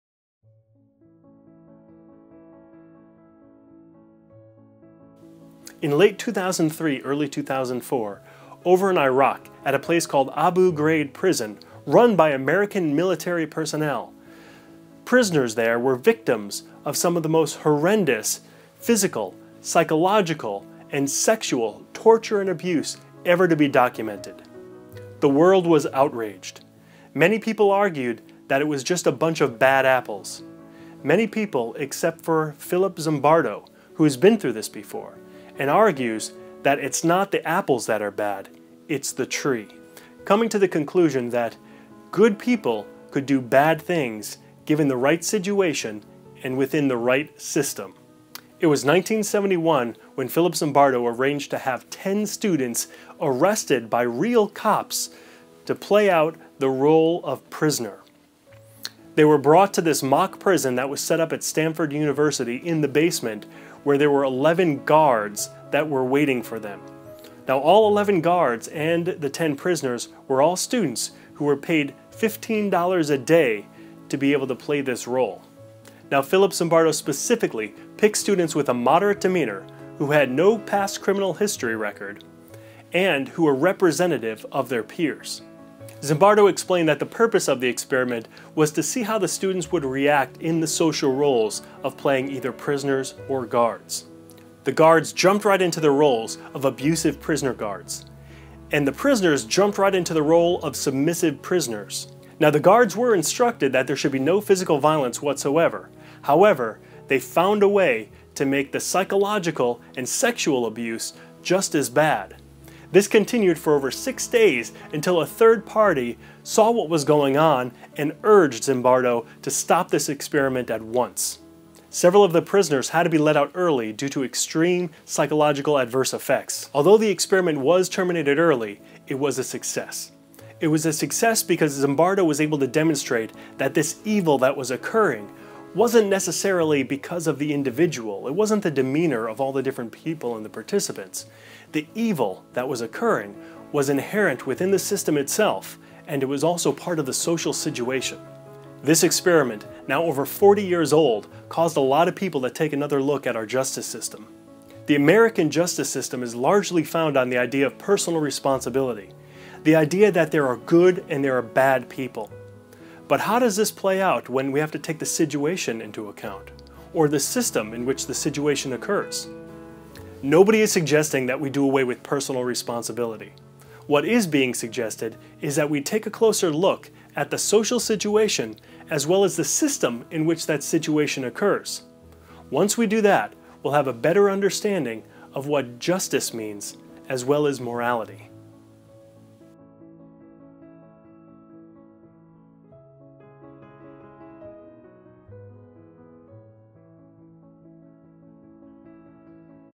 Stanford Prison Experiment
In the summer of 1971, the police publicly "arrested" 10 students who did nothing wrong besides agree to participate (for $15 a day) in one of social psychology's most notorious studies since Milgram's house of pain back in the 60s.  These students were taken to the "Stanford County Prison" (a mock prison located at the university) where they were set to be held prisoner for the next two weeks.